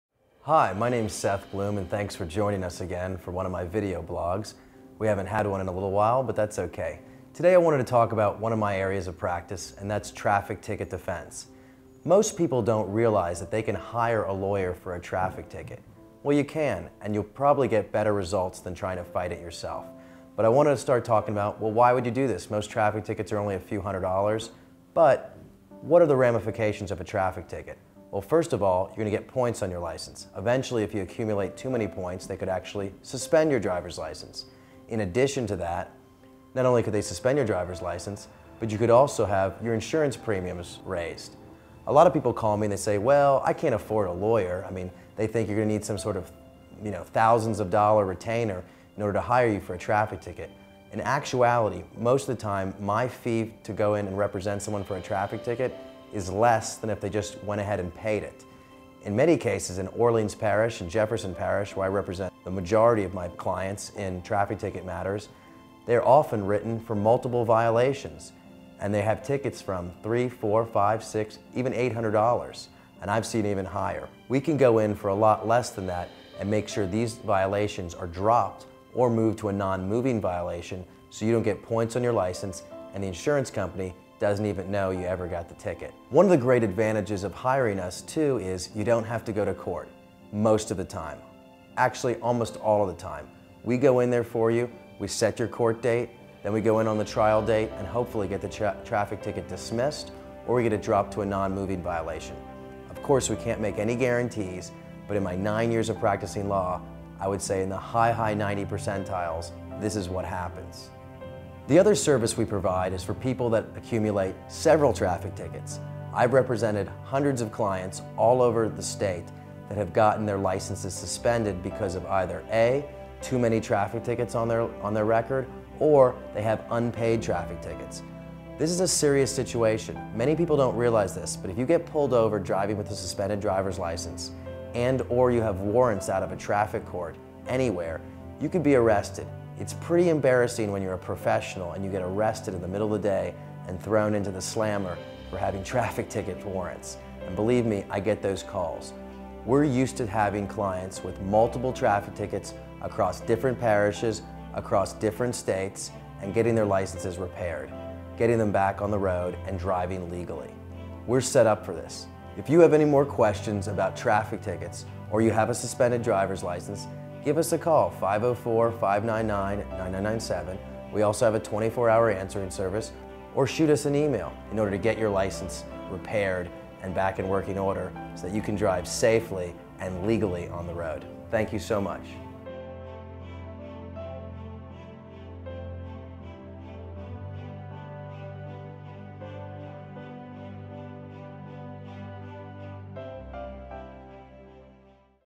BloomLegalTV: Seth Bloom, New Orleans Traffic Ticket Lawyers Stresses Importance of Fighting Tickets
New Orleans traffic ticket lawyer, Seth Bloom of Bloom Legal understands that traffic tickets may seem like a nuisance, or just an overall hassle to fight. However, Seth wants to remind his New Orleans drivers that traffic tickets, no matter how minor the infraction, can have a big impact. Call the New Orleans traffic ticket lawyers at Bloom Legal today to discuss how they can help you avoid costly insurance premiums and points on your license or even a license suspension.

700 Camp Street
New Orleans, Louisiana 70130